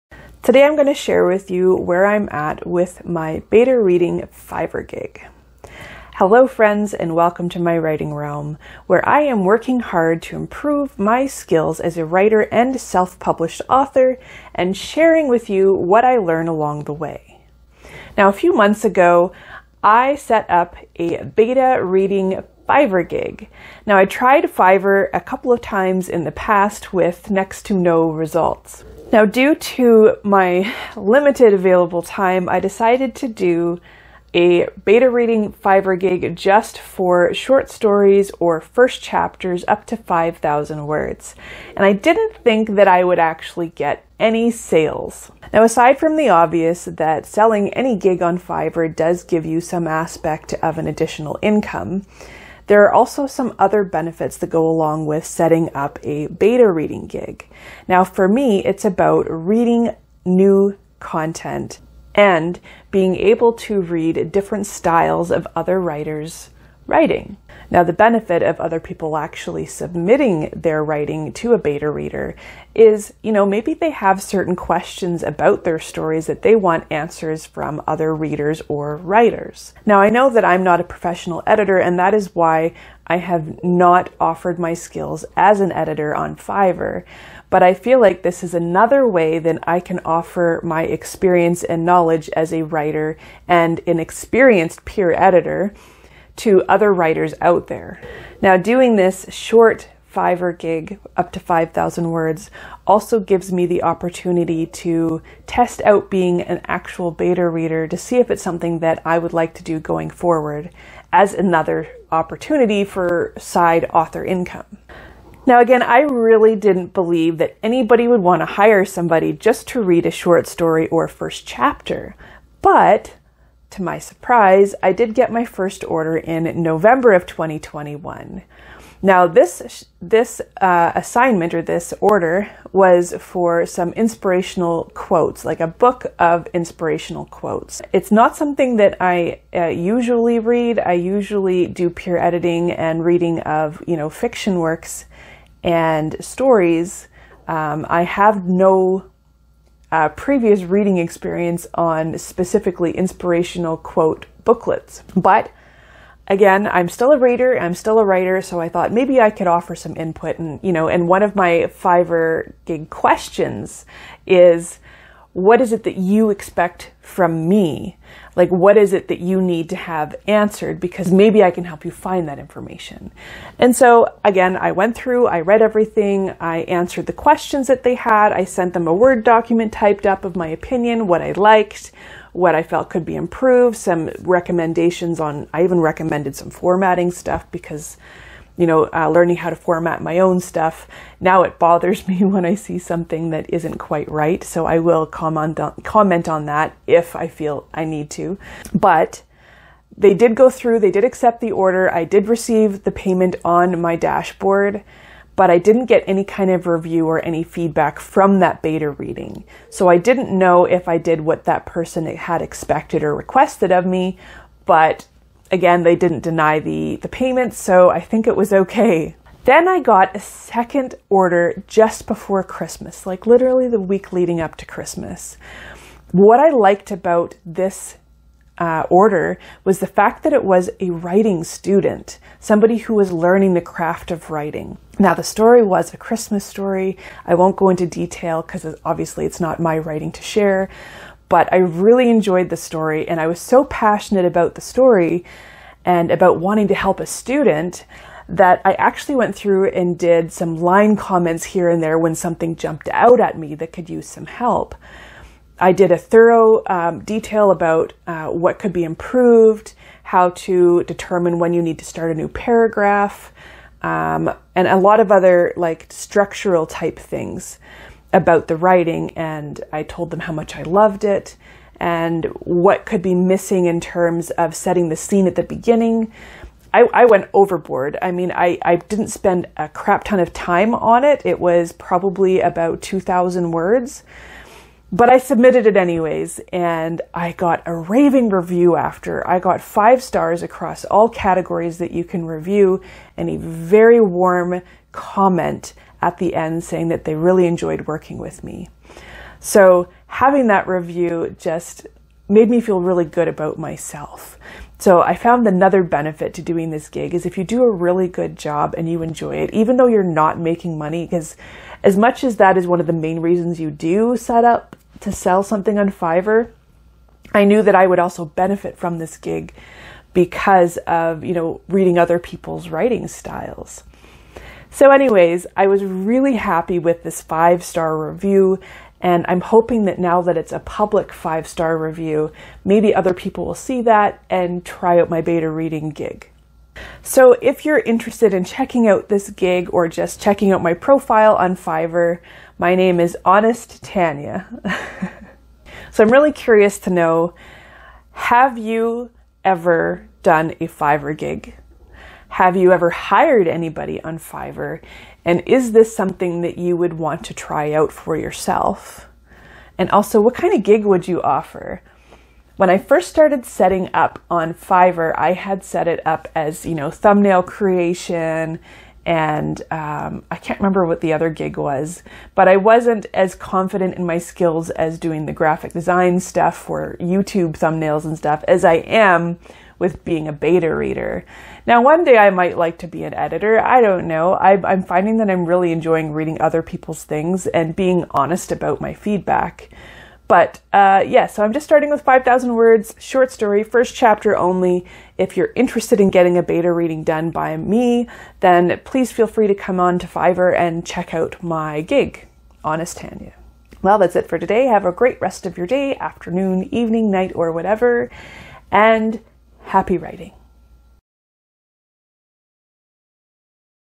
Beta Reader Fiverr Gig Update / 5 STAR REVIEW!
The first attempt at selling a gig on Fiverr did not go as planned. But this time, things seem to be looking up. I've had a Beta reading gig on Fiverr for a couple of months and finally got my first couple of sales.

Have you ever purchased or sold on Fiverr?